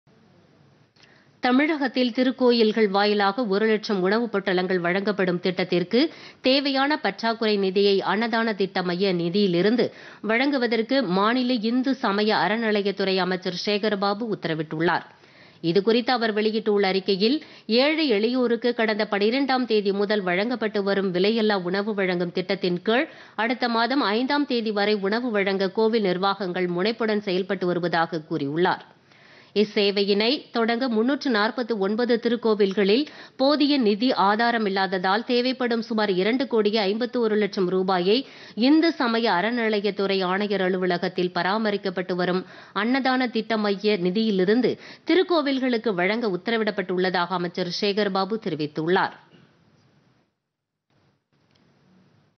அன்னதான திட்ட மைய நிதியிலிருந்து வழங்க உத்தரவு
தமிழகத்தில் திருக்கோயில்கள் வாயிலாக ஒரு லட்சம் உணவுப்பொட்டலங்கள் வழங்கப்படும் திட்டத்திற்கு தேவையான பற்றாக்குறை நிதியை அன்னதான திட்ட மைய நிதியிலிருந்து வழங்குவதற்கு மாநில இந்து சமய அறநிலையத்துறை அமைச்சர் சேகர்பாபு உத்தரவிட்டுள்ளார்.